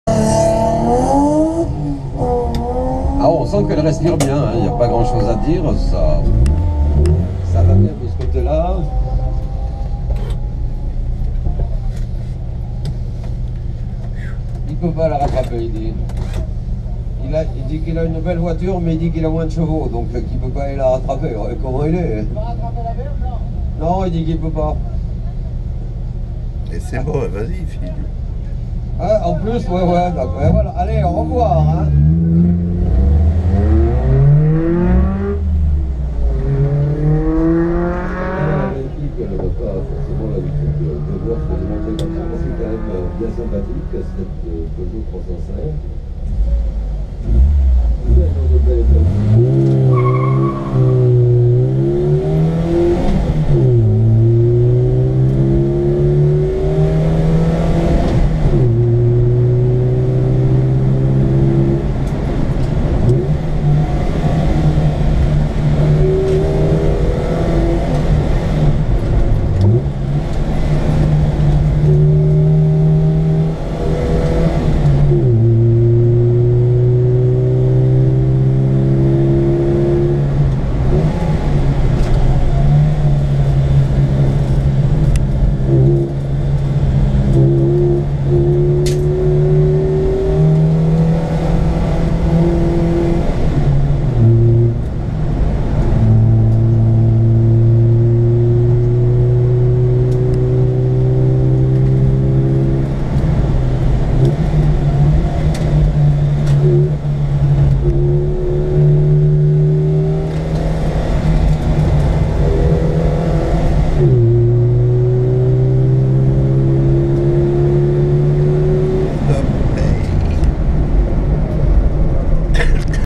Montée historique de Vélines 2015 305 GT (1)
Montée historique de Vélines 2015 en Peugeot 305 GT le 8 mai 2015